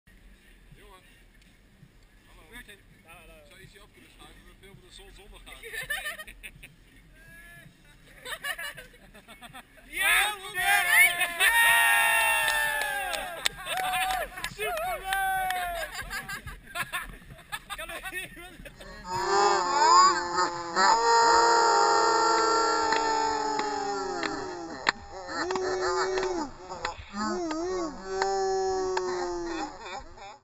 Superman saves the day at Val Thorens
Iemand verliest zijn snowboard op de piste gelukkig hangt een man in een Superman pak de held uit.